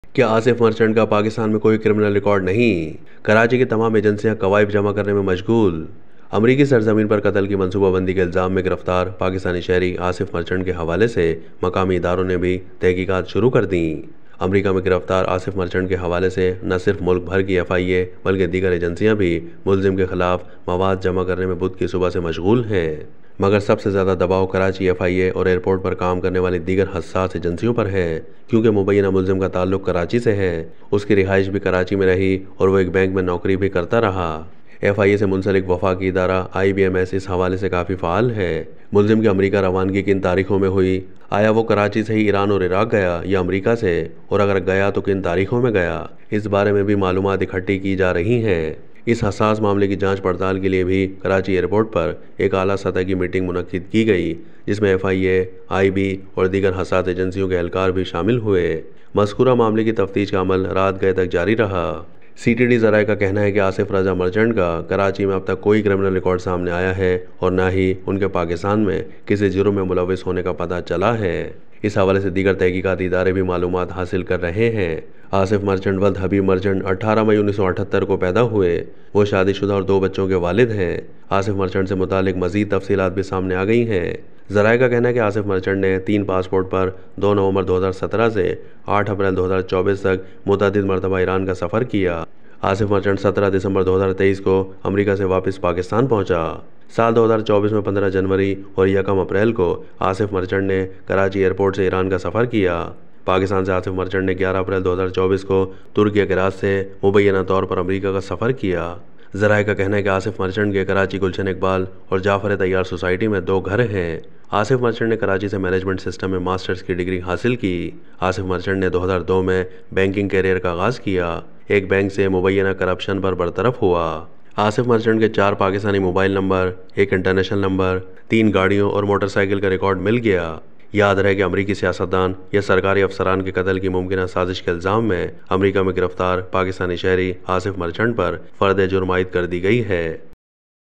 Asif Merchant has no criminal record in Pakistan?
Regarding the arrest of Asif Merchant in the USA, not only the FIA across the country but also other agencies are busy collecting evidence against the accused. But the most pressure is on Karachi FIA.